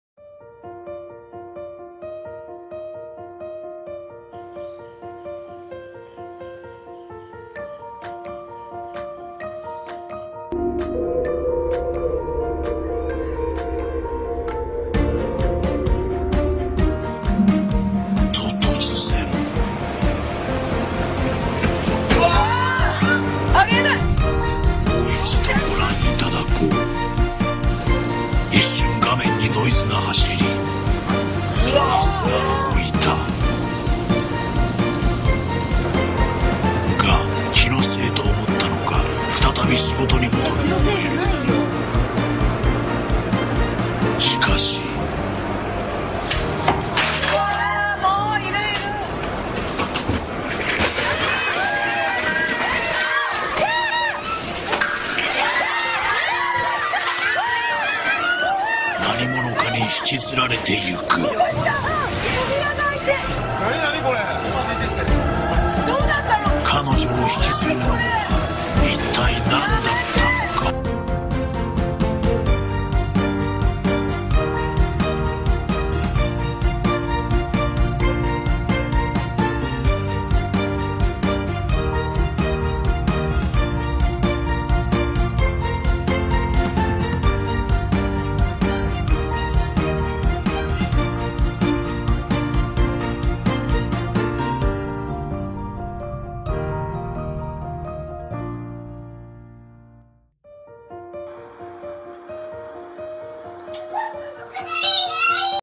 100% Real ! Alien Abduction caught on tape
Alien abduction or poltergeist activity?or the two are the same ?!!Thats what I think.Abduction caught on camera!Invisible alien pulls the victim by Her leg